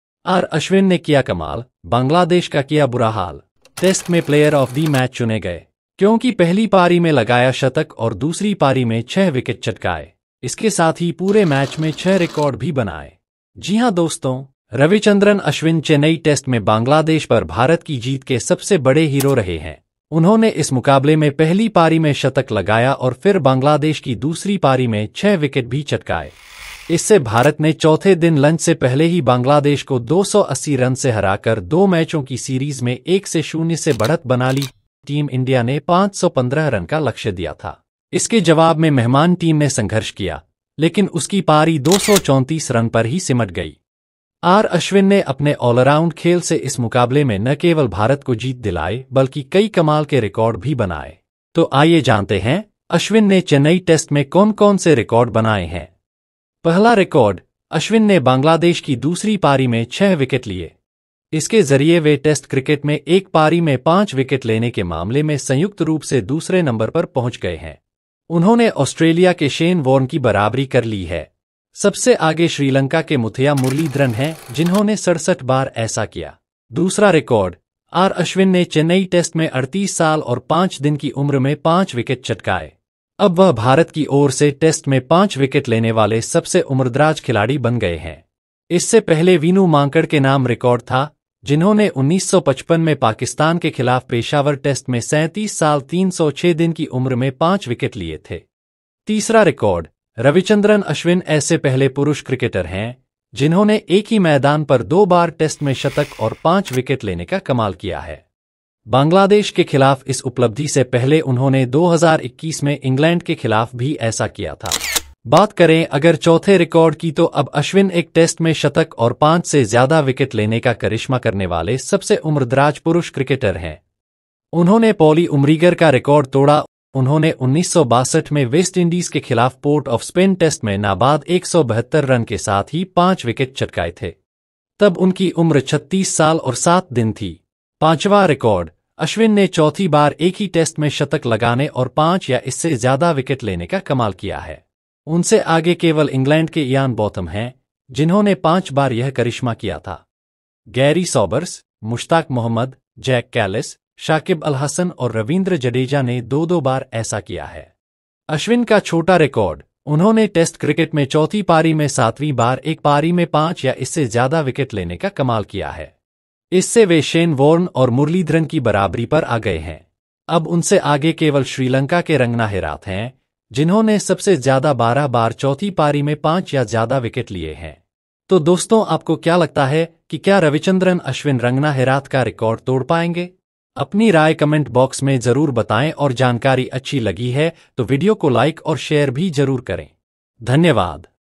🏏R. Ashwin's Record-Breaking Performance: Crush Bangladesh | Must Watch
🏏R. Ashwin's Record-Breaking Performance: Crush Bangladesh | Must Watch | 🏏R. Ashwin's Record-Breaking Masterclass: Century & 6 Wickets Crush Bangladesh

About This Video: India's star all-rounder Ravichandran Ashwin once again showcased his brilliance in the Chennai Test against Bangladesh! 🏏🔥 With an incredible century in the first innings and a six-wicket haul in the second, Ashwin led India to a massive 280-run victory. Not only was he named Player of the Match, but he also shattered SIX major records during this thrilling match! 🏆

In this video, we dive into Ashwin's match-winning performance and the historical milestones he achieved: 1️⃣ Equals Shane Warne with the most 5-wicket hauls in Tests! 2️⃣ Becomes the oldest Indian to claim a 5-wicket haul in a Test. 3️⃣ First male cricketer to score a century and take 5 wickets on the same ground—twice! 4️⃣ Sets a new record as the oldest to achieve a century and 5 wickets in a Test. 5️⃣ Ties with Ian Botham for the most Tests with 100+ runs and 5+ wickets. 6️⃣ Matches Muralitharan & Warne with 7 five-wicket hauls in 4th innings.

Can Ashwin surpass Rangana Herath’s record for most 5-wicket hauls in the 4th innings? 👇